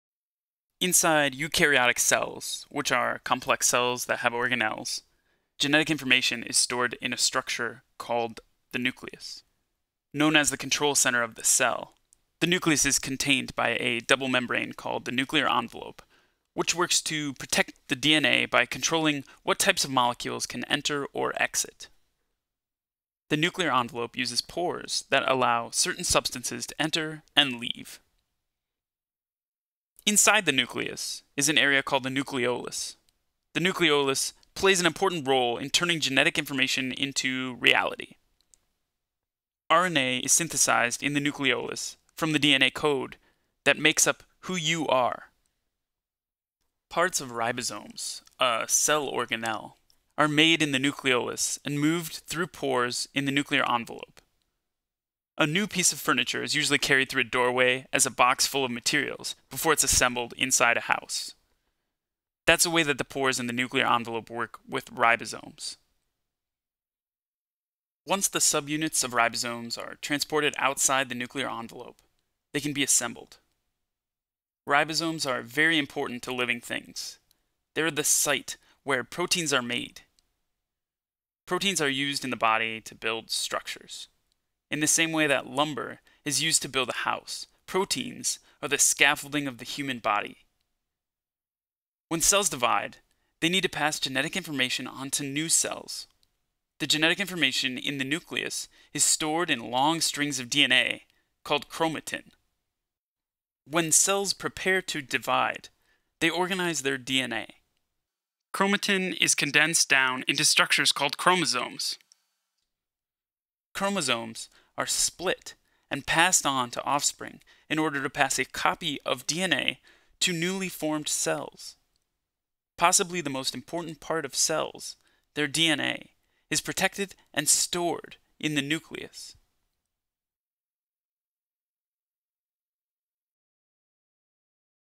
Nucleus
This video describes the structure and function of the cell nucleus.